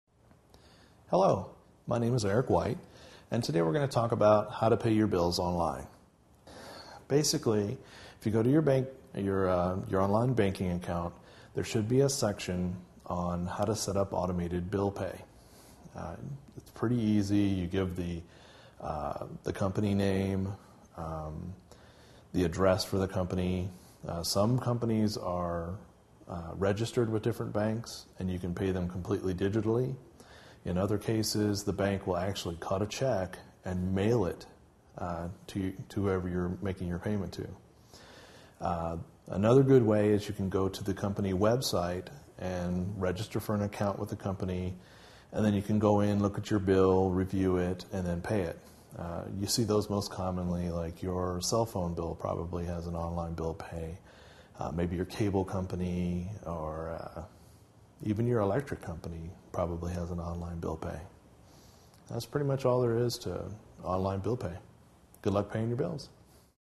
How to Pay Bills Online
How to Pay Bills Online. Part of the series: Computers & Internet: Part 1. Pay bills online by setting up automated bill pay using an online banking account. Various companies, such as cable or utility companies, may also offer online bill payment. Pay bills online with tips from an experienced computer user in this free video on using the Internet.